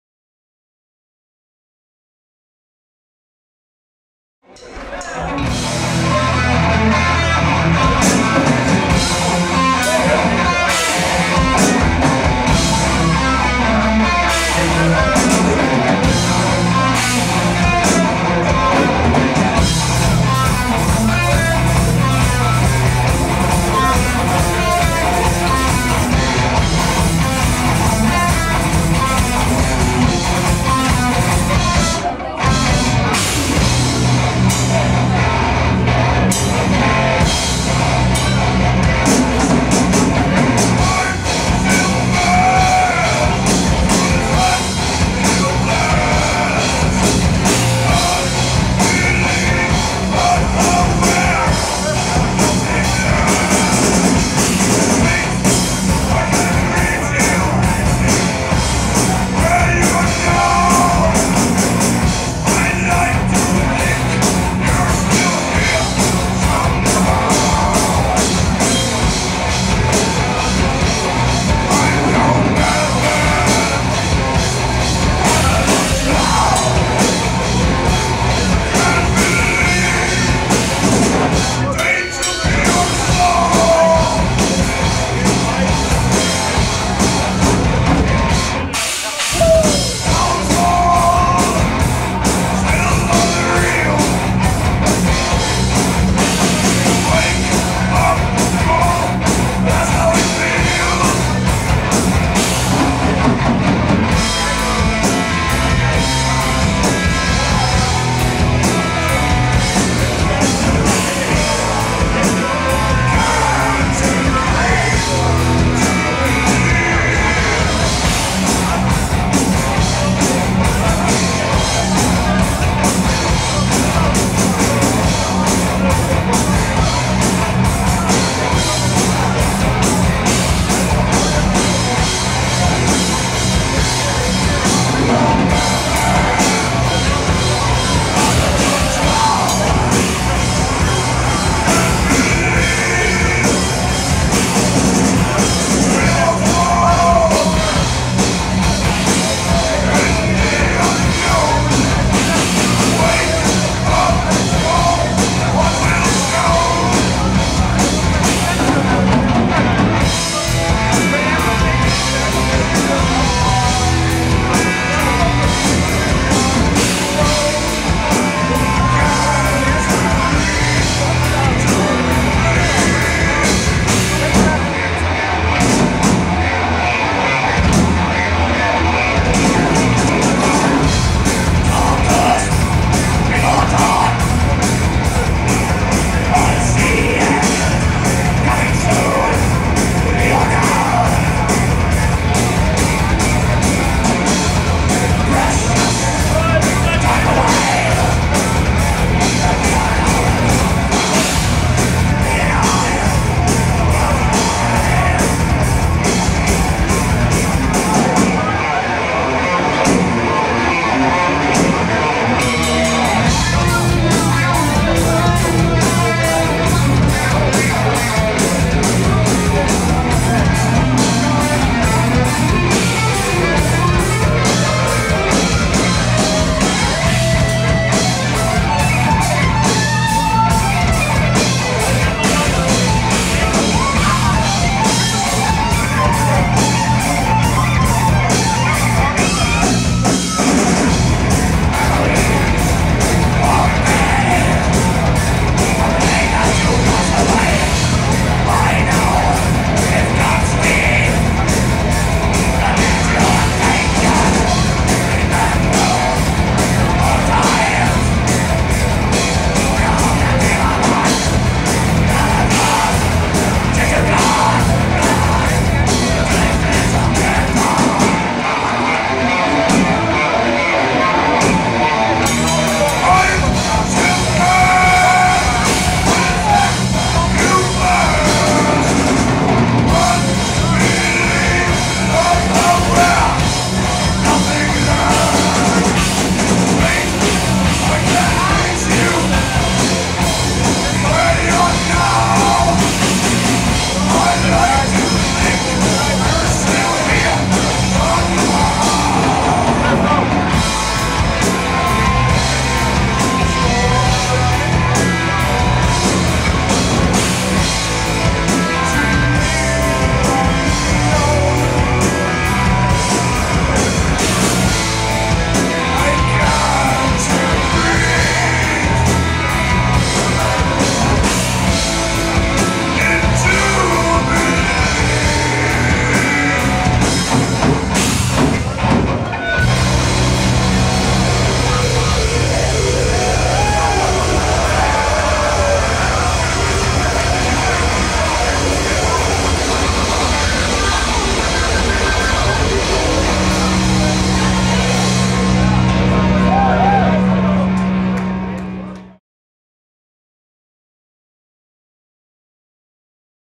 Dead Man's Walk - To Hell Fest VI - O.J.C. Maddogs Groesbeek
Dead Man's Walk is a metal band from Nijmegen, the Netherlands. This is recorded on Saturday 12 December 2009 at "To Hell Fest VI" in O.J.C. Maddogs Groesbeek, the Netherlands.